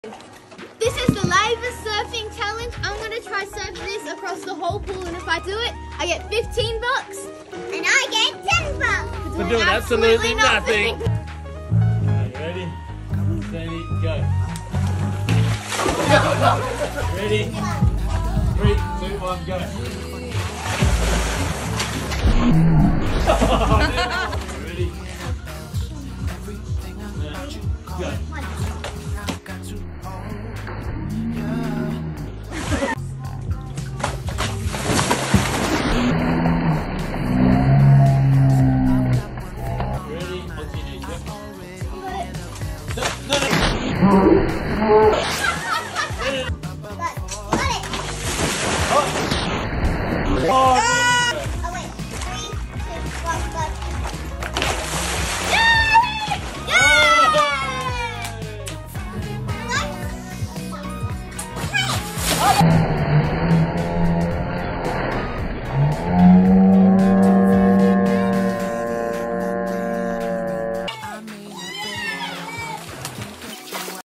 New pool challenge!
It’s a hot summer in Perth of course it’s time for a new pool challenge!